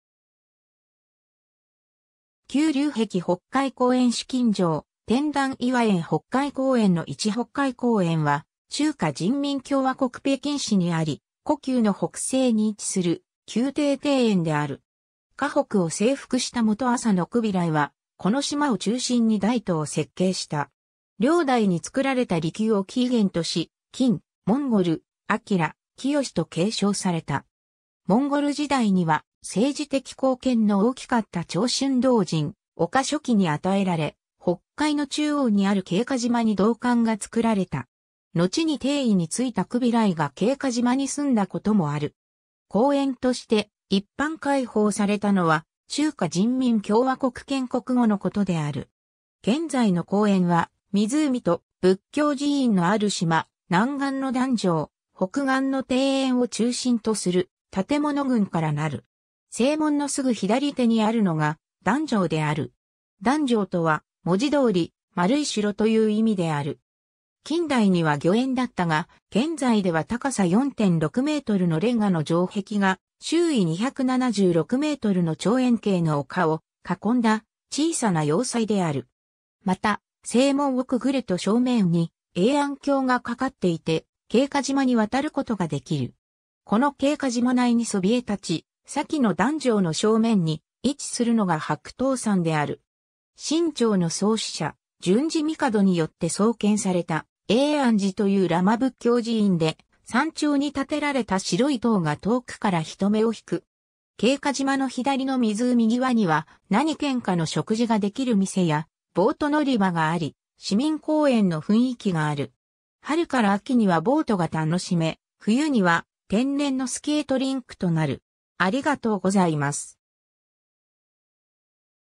北海公園
九龍壁 北海公園 紫禁城 天壇 頤和園 北海公園の位置（北京市） 北海公園（ほっかいこうえん、簡体字中国語: 北海公园、拼音: Běihǎi Gōngyuán）は中華人民共和国北京市にあり、故宮の北西に位置する宮廷庭園（旧皇帝御園）である。
華北を征服した元朝のクビライは、この島を中心に大都を設計した。
 遼代に作られた離宮を起源とし、金、モンゴル、明、清と継承された。
モンゴル時代には政治的貢献の大きかった長春道人・丘処機に与えられ、北海の中央にある瓊華島に道観（道教の寺院）が作られた。
のちに帝位についたクビライが 瓊華島に住んだこともある。
公園として一般開放されたのは、中華人民共和国建国後のことである。
 現在の公園は湖（北海）と仏教寺院のある島、南岸の団城、北岸の庭園を中心とする建物群からなる。
正門のすぐ左手にあるのが、団城である。
団城とは、文字通り「丸い城」という意味である。
金代には御苑だったが、現在では高さ4.6メートルの煉瓦の城壁が周囲276メートルの長円形の丘を囲んだ小さな要塞である。
また、正門をくぐると正面に永安橋が架かっていて瓊華島に渡ることができる。
この瓊華島内にそびえたち、先の団城の正面に位置するのが白塔山（万寿山）である。
清朝の創始者・順治帝によって創建された永安寺というラマ仏教寺院で、山頂に建てられた白い塔が遠くから人目をひく。
瓊華島の左の湖ぎわには、何軒かの食事ができる店やボート乗り場があり、市民公園の雰囲気がある。
春から秋にはボートが楽しめ、冬には天然のスケートリンクとなる。